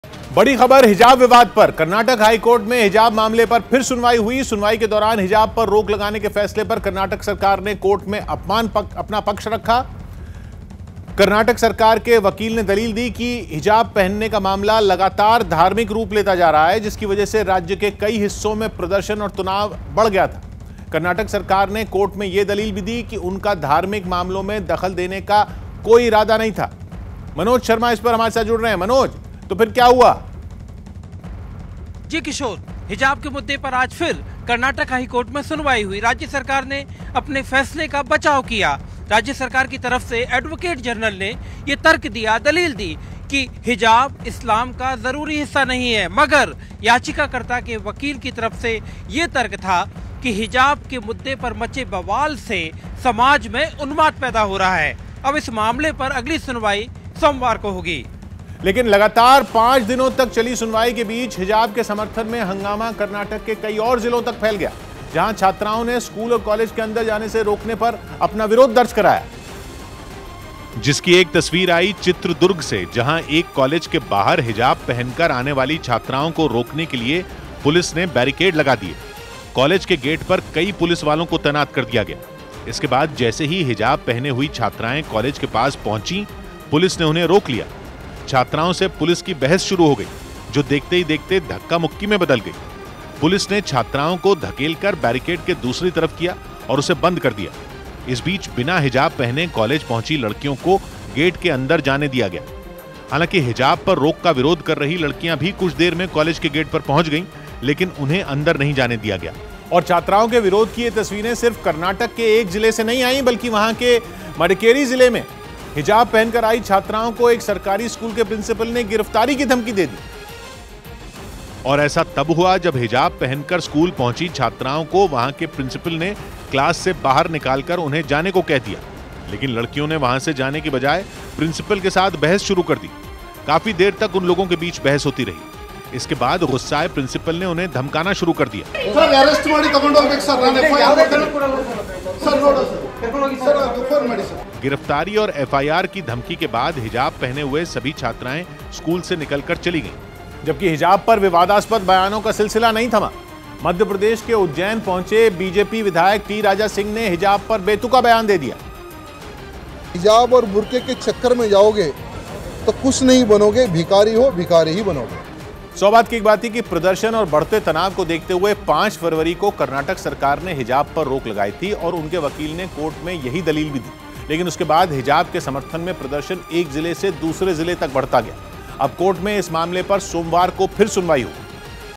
Hijab Controversy सोमवार को HC में फिर सुनवाई, हिजाब पर रोक का विरोध
Hijab Issue : हिजाब मामले को लेकर Karnataka High Court में सुनवाई हुई.कर्नाटक से शुरू हुआ हिजाब विवाद देशभर में फ़ैल चुका है.जगह जगह हिजाब के नाम पर विरोध प्रदर्शन हो रहा है.